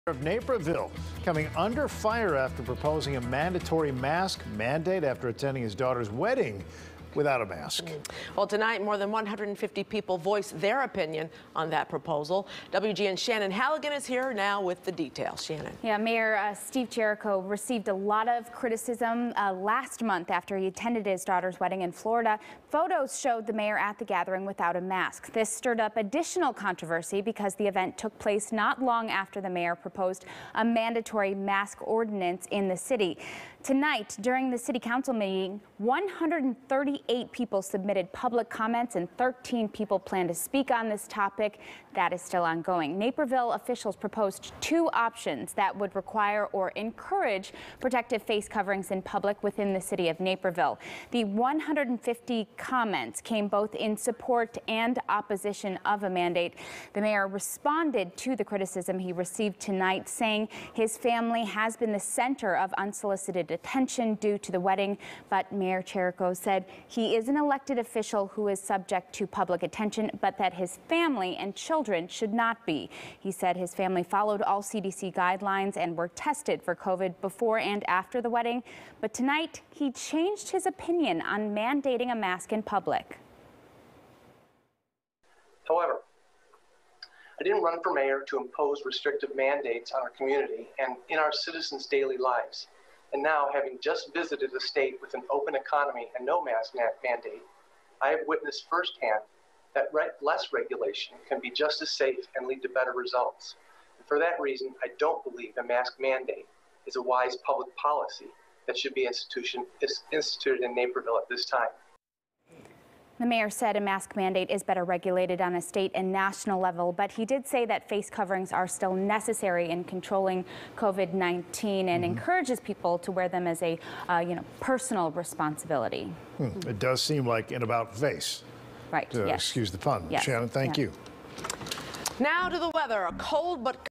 Naperville mayor changes opinion on masks after proposing mandate
Naperville Mayor Steve Chirico has changed his opinion on masks after proposing a mandatory mask ordinance in the city.